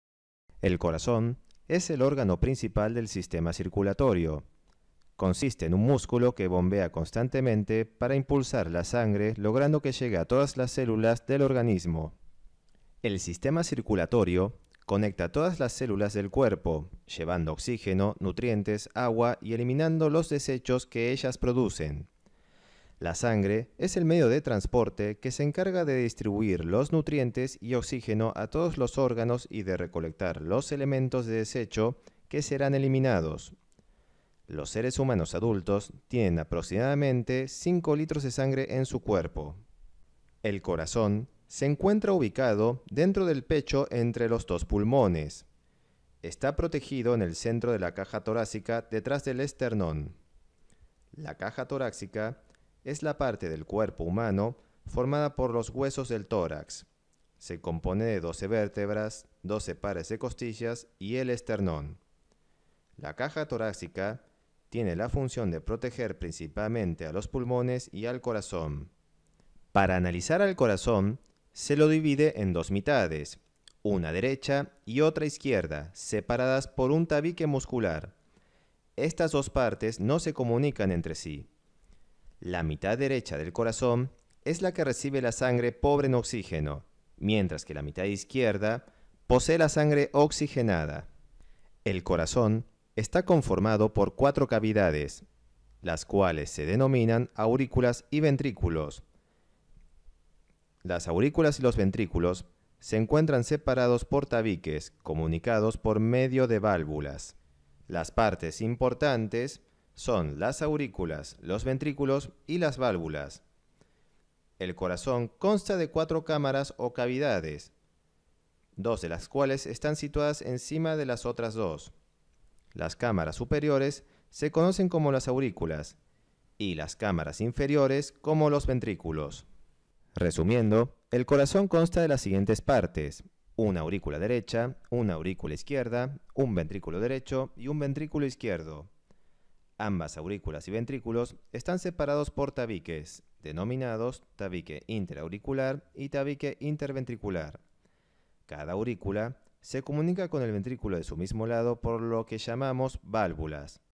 El corazon y sus partes
Este vídeo es la primer parte sobre como funciona el corazón en el sistema circulatorio

Enlace